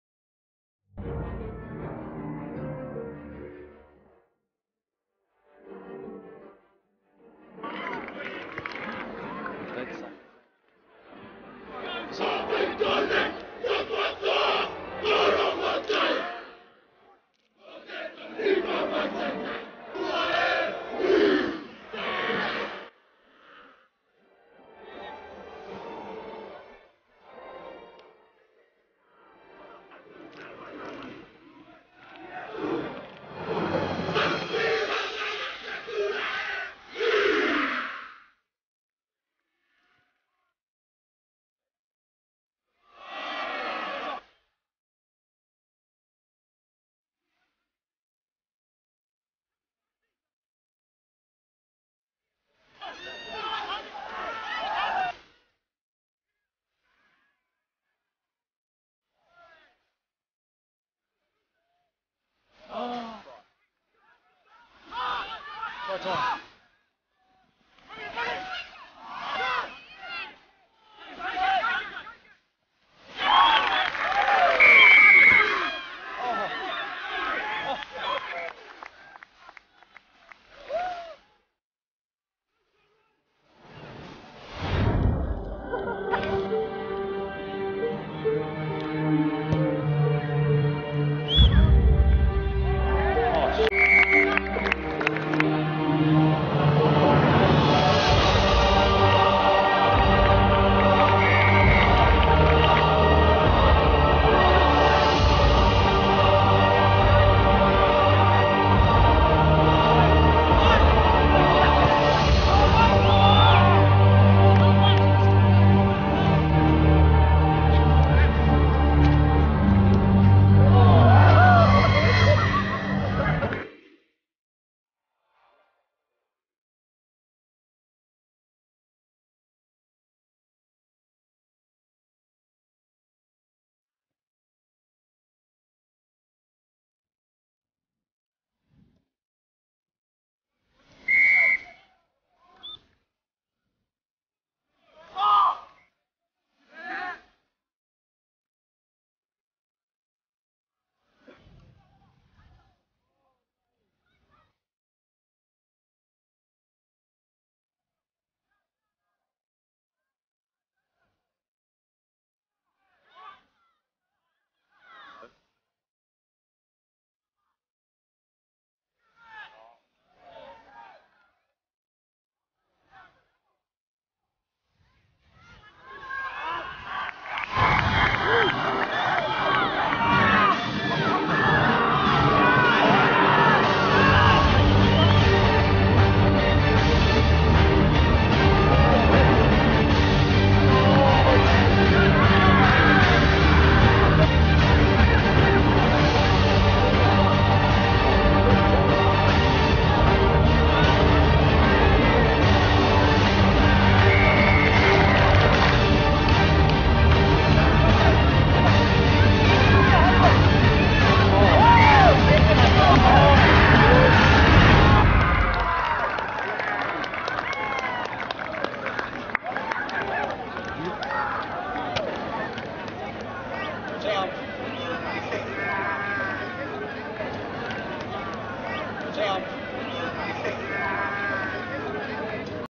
Rongotai College 1st XV v St Pats Silverstream 1st XV | 2010 | Traditional
Rongotai wins 19-13 over St Pats Silverstream in the Annual Traditional